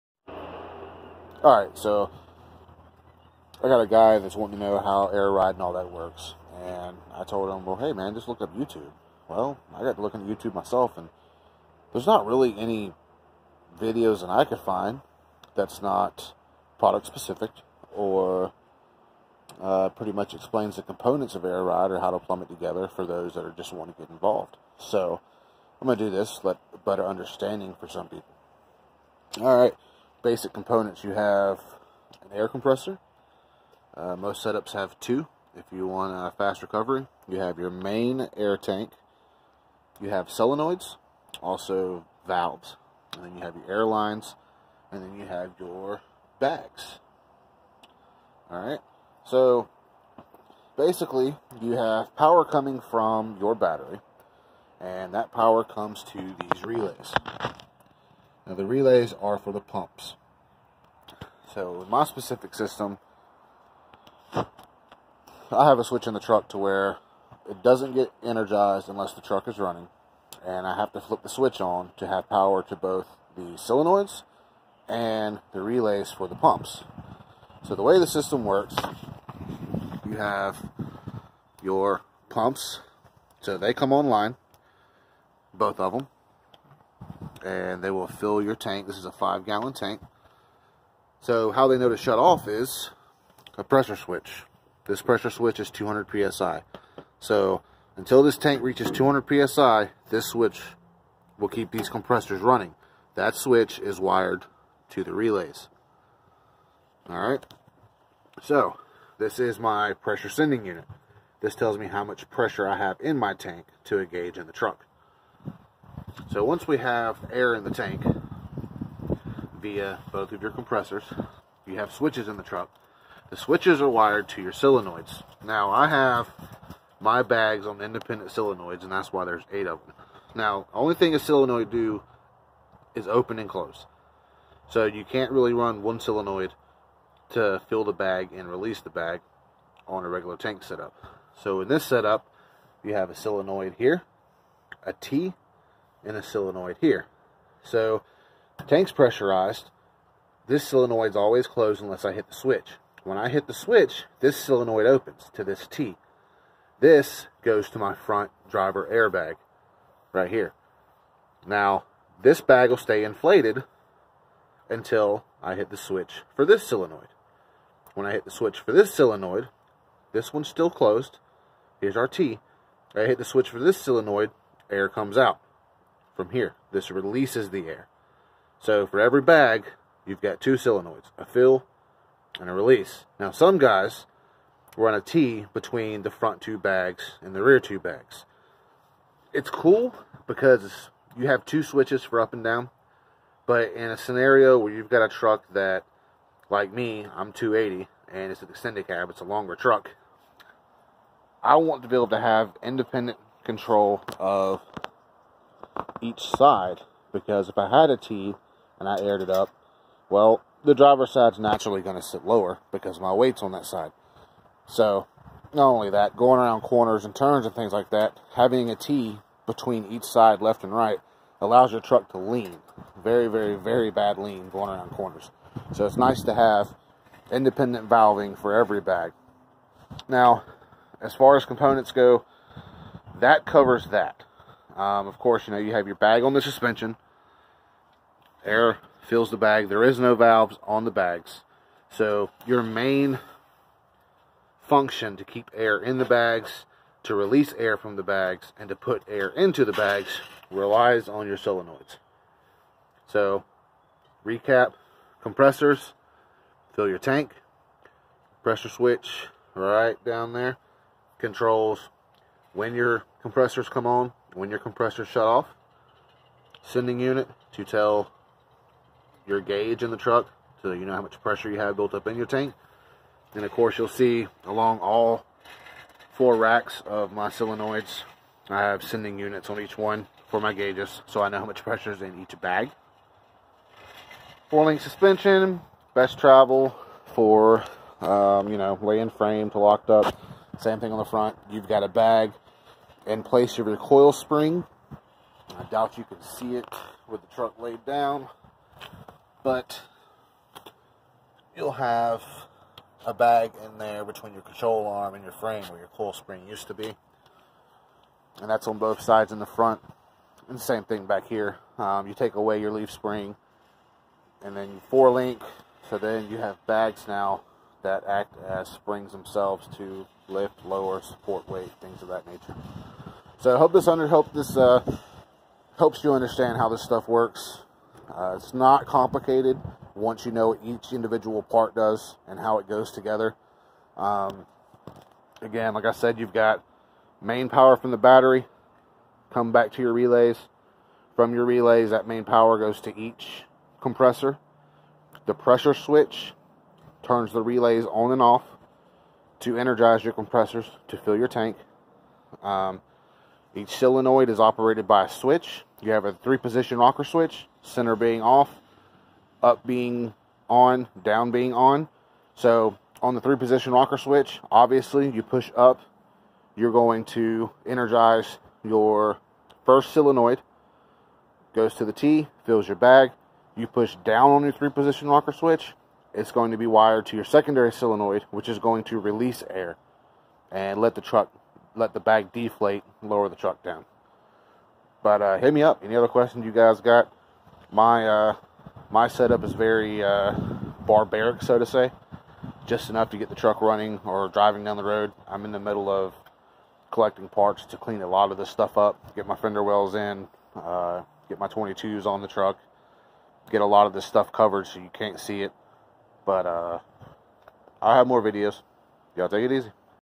The basics of air ride. Components, plumbing, electrical.
Short version of those wanting to get I to air ride suspensions but, don't know where to start. In this video I cover each individual part, it's function, how it's energized, it's plumbing, and it's purpose. This isn't some super technical in depth "how's it work" video, just a basic guide for the beginner in understanding how it all works.